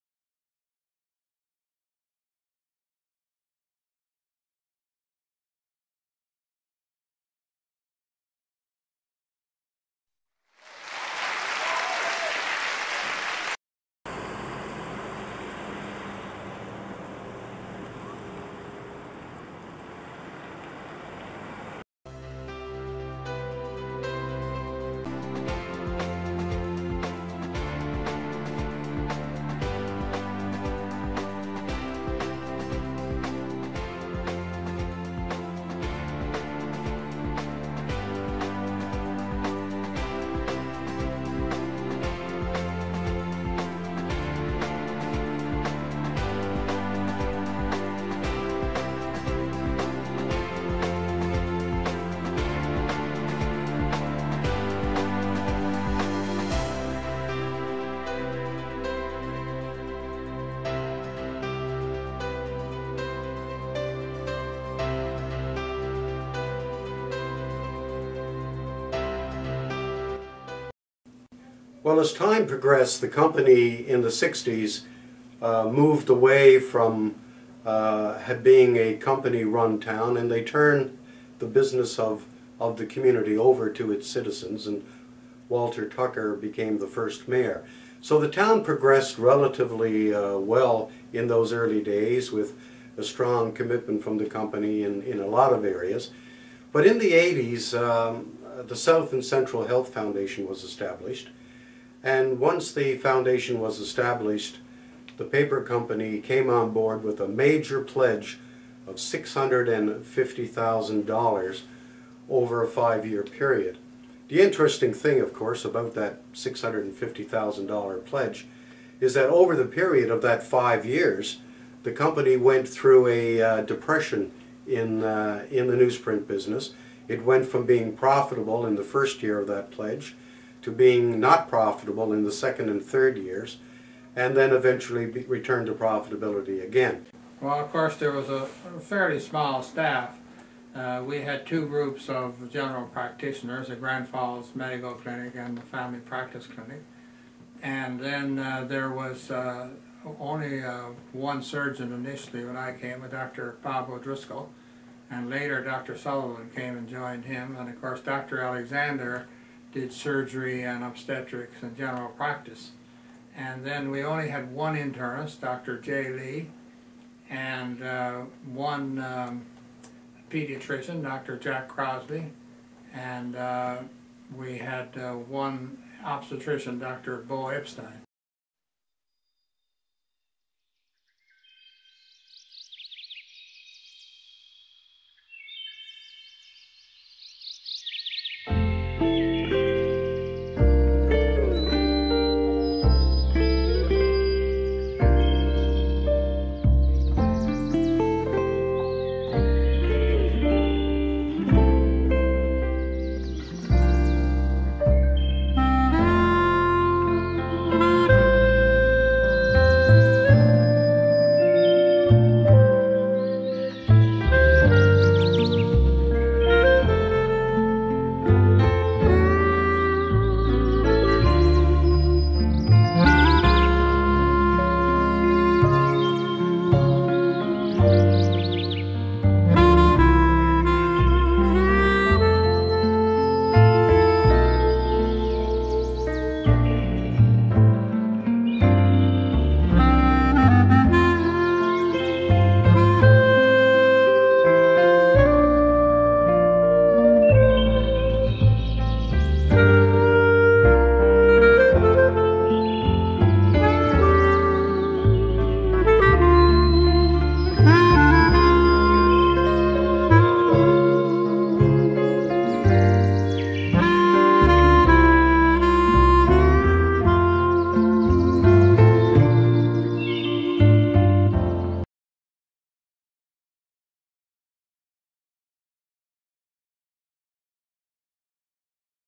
Central Newfoundland Hospital's 50th Anniversary!!!! 1963-2013
Fifty years of Healthcare takes you back to 1906 when the need for healthcare services first occurred during the construction of the Mill in Grand Falls. Video is made up of interviews and historic pictures from the early days of the Central Newfoundland Hospital. Video is 44:54 with a bonus slideshow of pictures and music.